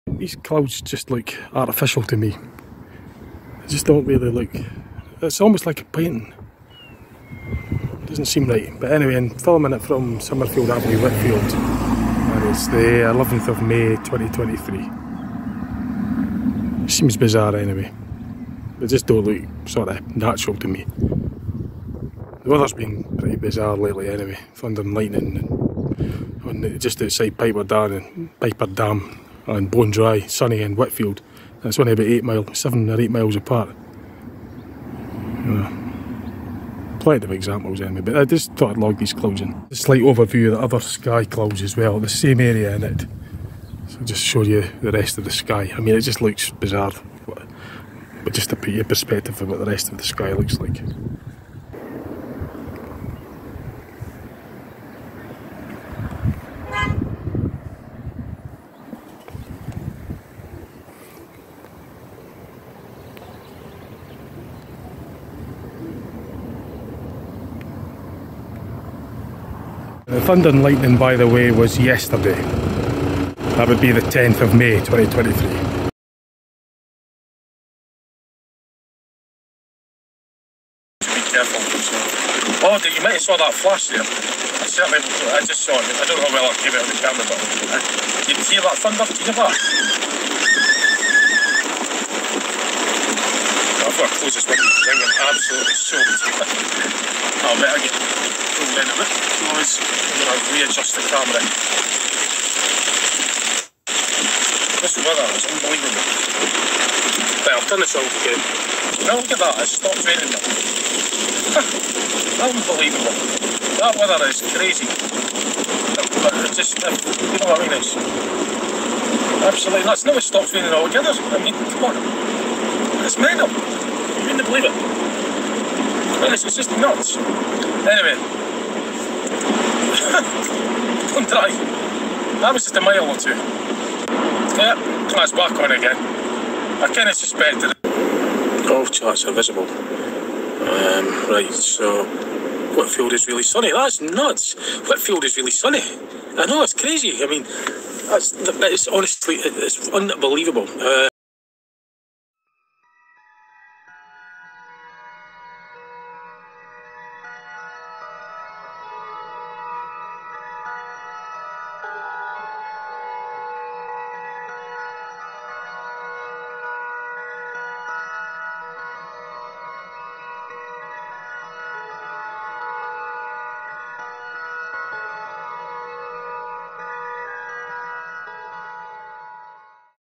Geoengineering ???(see link in description) .....Scotland....2023....
My footage I taken of weather phenomenon I find strange.

I've included a link below, to a video I found interesting about Geoengineering :-